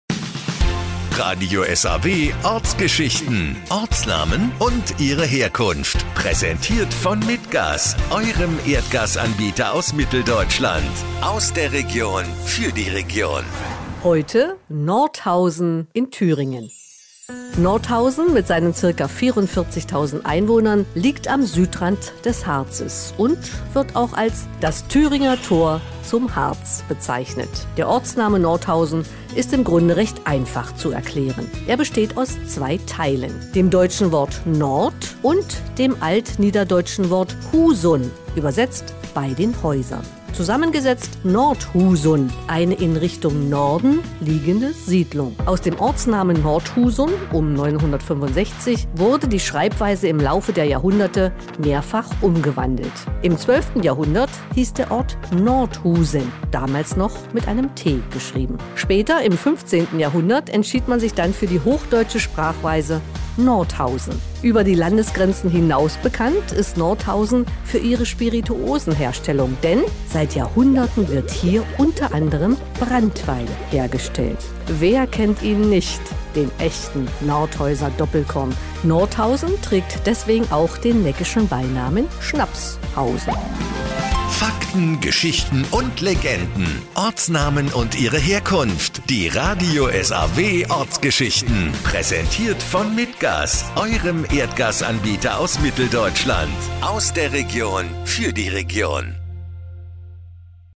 Nordhausen
Nordhausen mit seinen ca. 44.000 Einwohnern liegt am Südrand des Harzes und wird auch als „Das Thüringer Tor zum Harz“ bezeichnet. Der Ortsname Nordhausen ist im Grunde recht einfach zu erklären. Er besteht aus zwei Teilen. Dem deutschen Wort „Nord“ und dem altniederdeutschen Wort „husun“ …übersetzt „bei den Häusern“. Zusammengesetzt:  „Nordhusun“ … eine in Richtung Norden liegende Siedlung. Aus dem Ortsnamen Northusun um 965, wurde die Schreibweise im Laufe der Jahrhunderte mehrfach umgewandelt. Im 12. Jahrhundert hieß der Ort Northusen, damals noch mit einem „t“ geschrieben. Später im 15. Jahrhundert entschied man sich dann für die hochdeutsche Sprache und aus Northusen wurde Nordhausen. Über die Landesgrenzen hinaus bekannt ist Nordhausen für seine Spirituosenherstellung, denn seit Jahrhunderten wird hier u.a. Branntwein hergestellt. Wer kennt ihn nicht den „Echten Nordhäuser Doppelkorn“? Nordhausen trägt deswegen auch den neckischen Beinamen „Schnapshausen“!